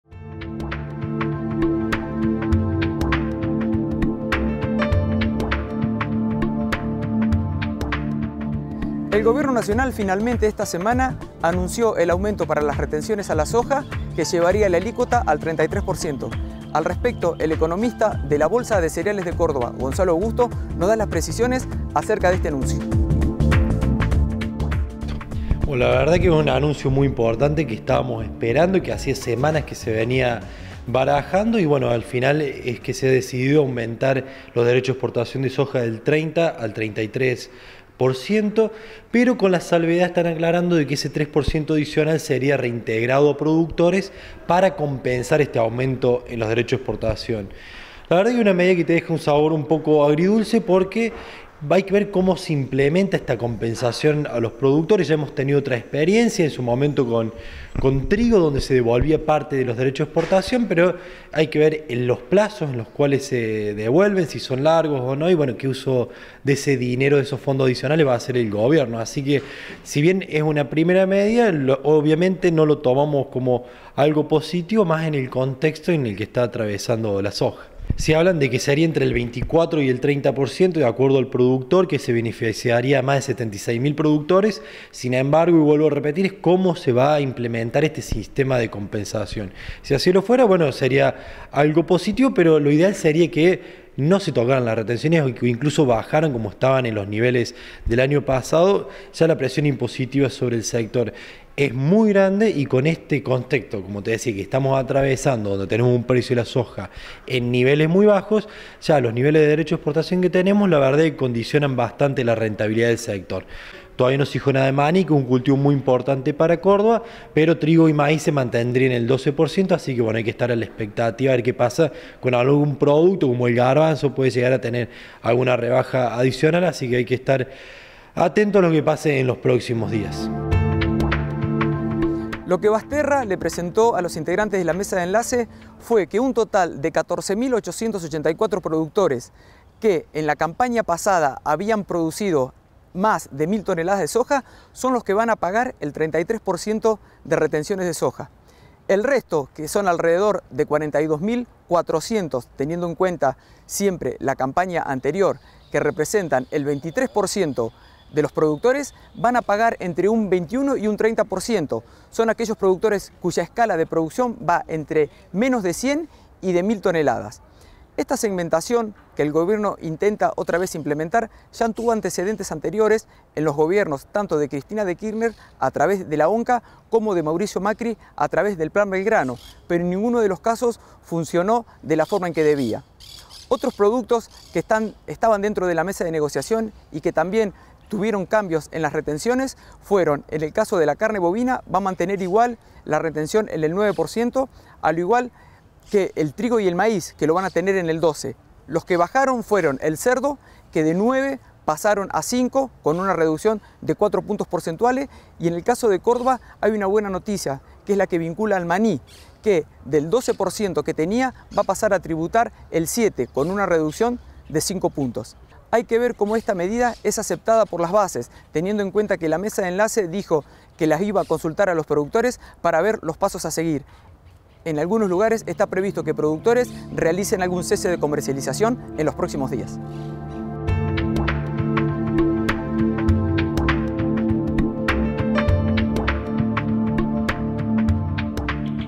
Más retenciones a la soja ¿Y ahora?
Finalmente el gobierno subió las retenciones a la soja en un 33 por ciento con algunas compensaciones. ¿Cómo afectará a la actividad agropecuaria?